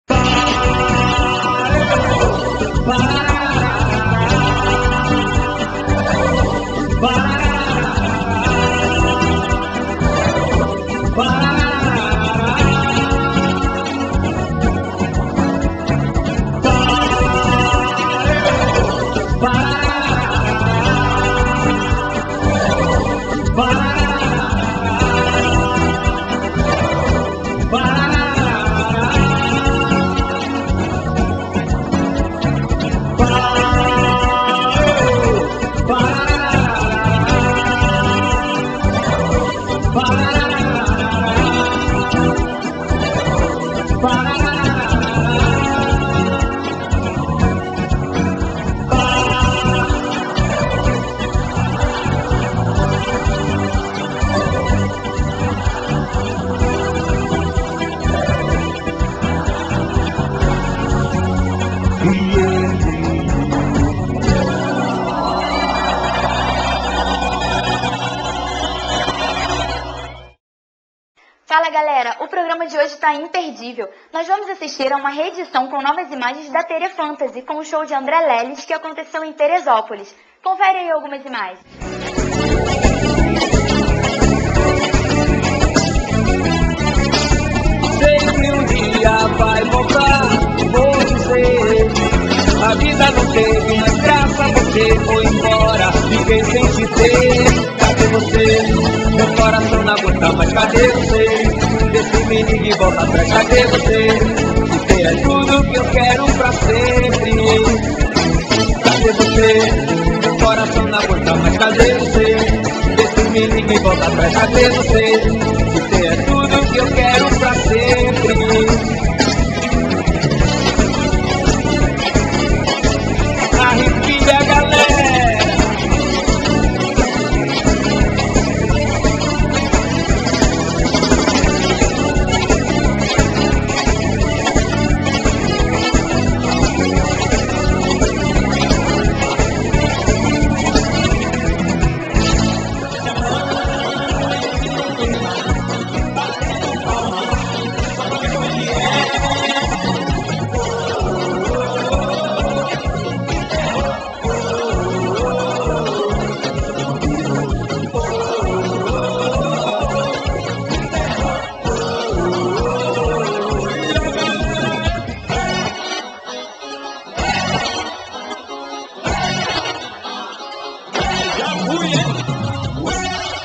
pgm 278.wmv
REEDIÇÃO, COM NOVAS IMAGENS, DA TERÊ FANTASY, COM O SHOW DO ANDRÉ LÉLIS, EM TERESÓPOLIS- RJ.